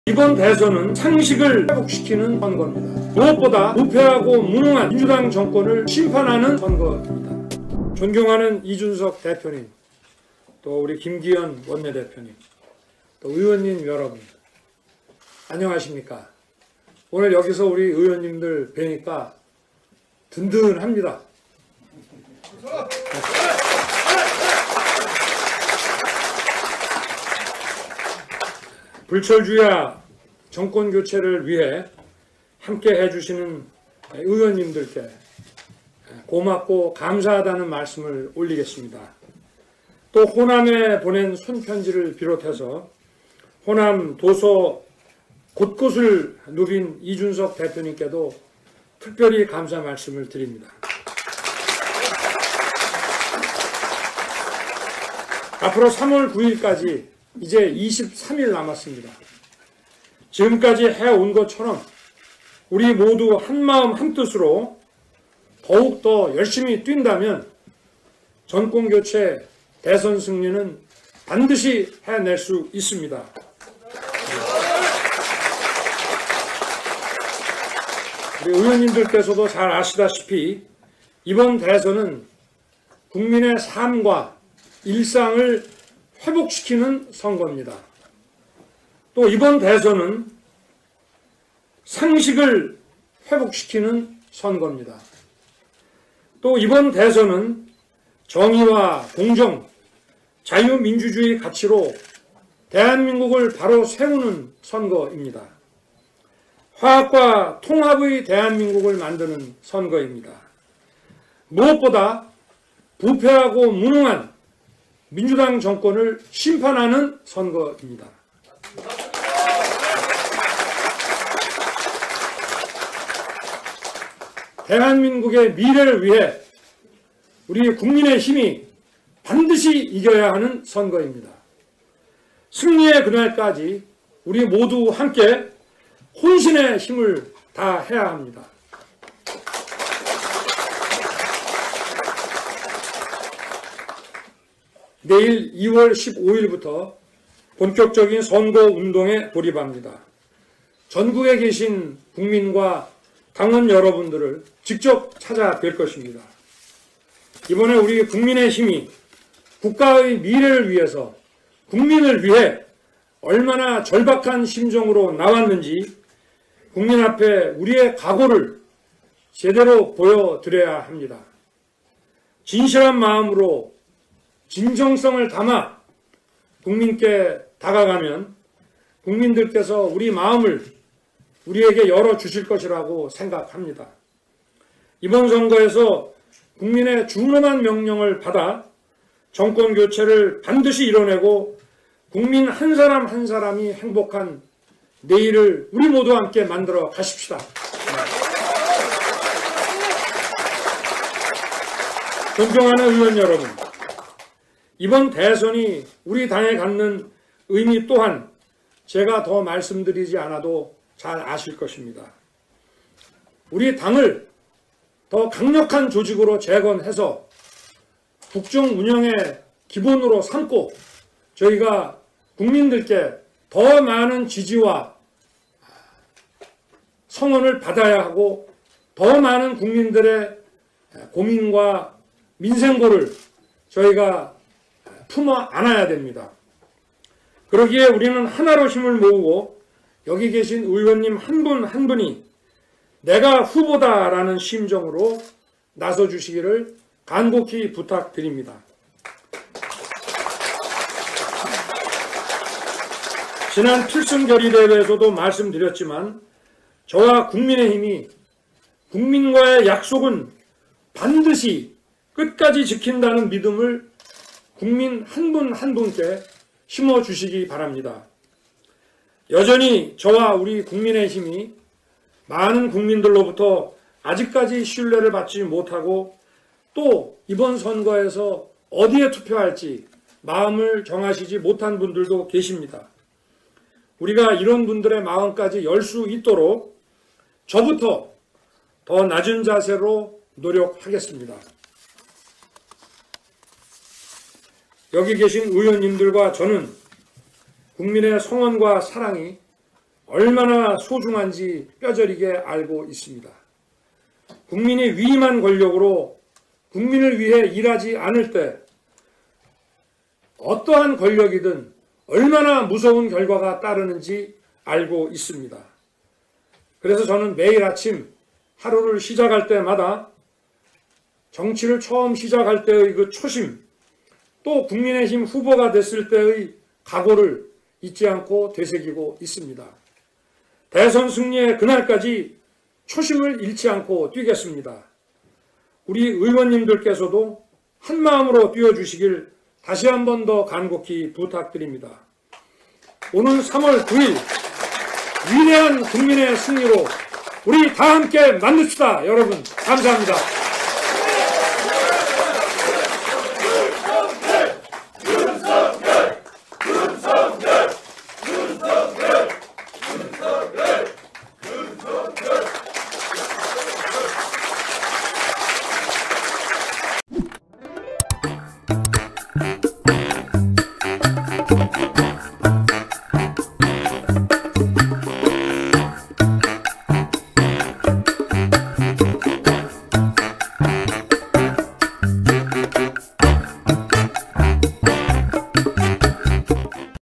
윤석열 "이번 대선은 상식을 회복시키는 선거…승리에 그날까지 초심 잃지 않고 뛸 것"
윤석열 국민의힘 대선 후보가 22년 2월 14일 서울 여의도 국회에서 열린 국민의힘 의원총회에서 발언하고 있다.